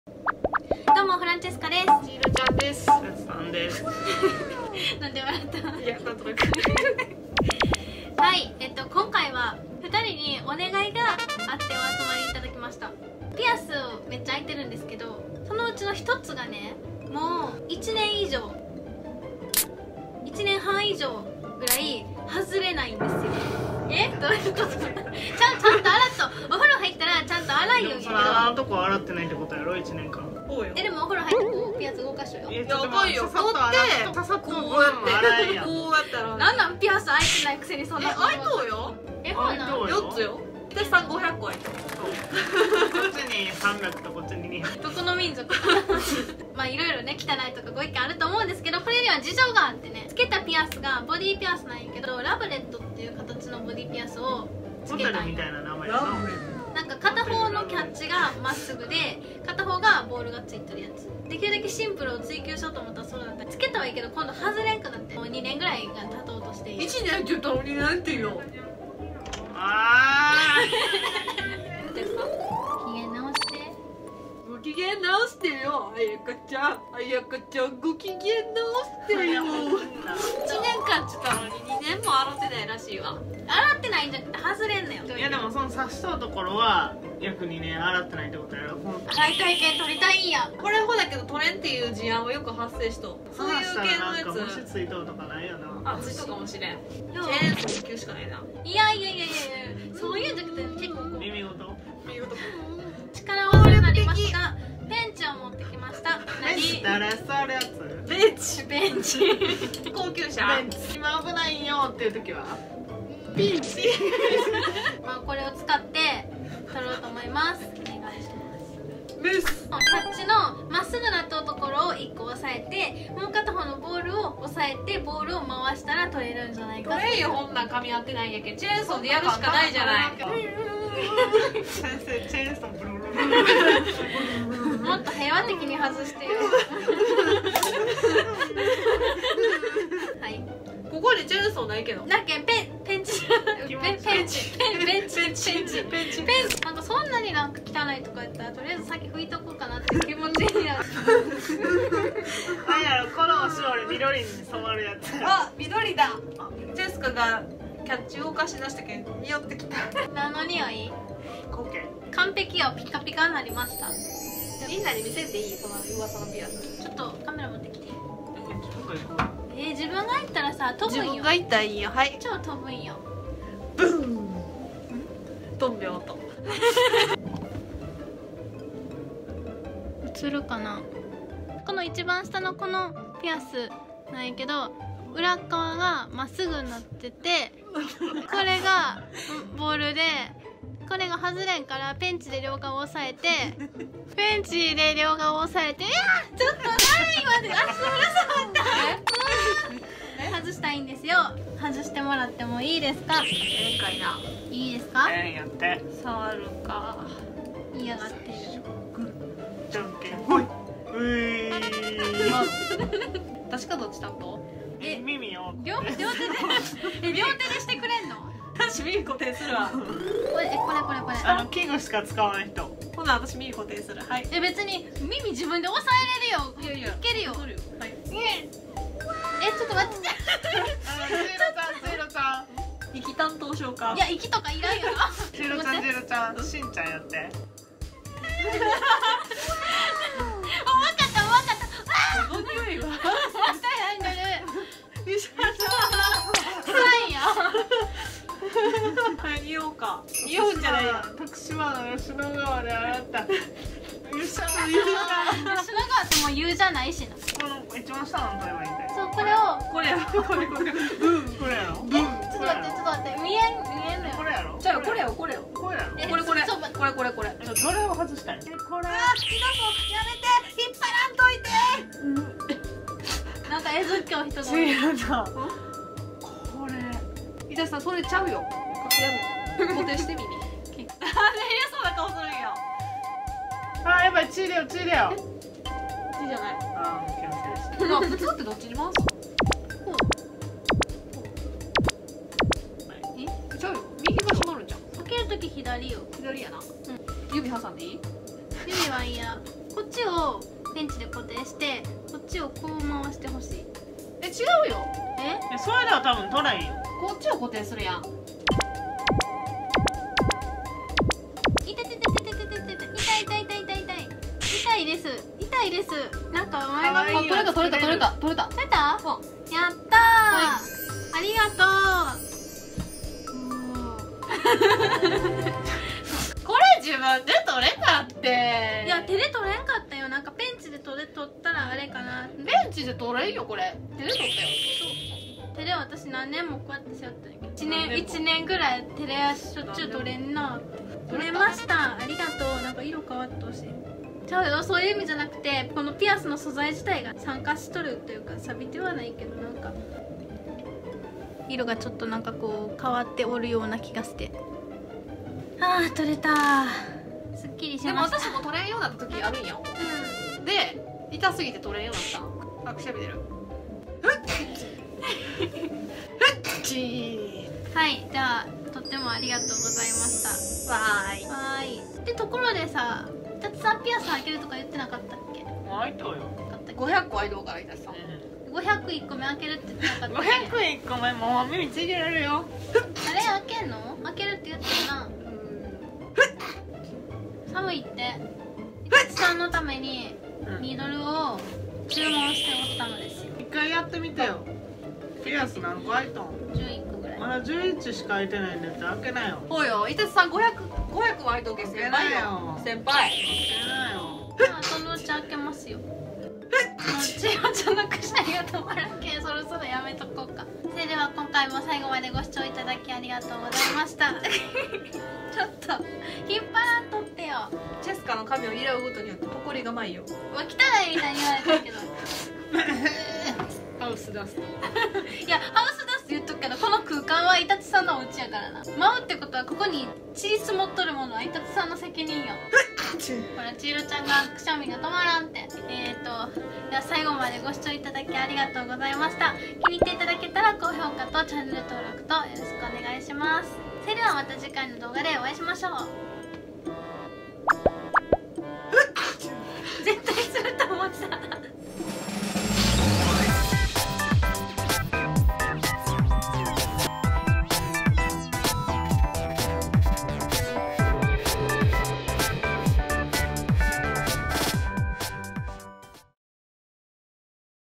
ピアスが外れなくなったのでペンチで強引に外そうとした結果
フランチェスカです(=^ω^=)モミモミ

今回はずっと外れなかったピアスを協力して外してもらいました。(そんなに臭くないよ 笑)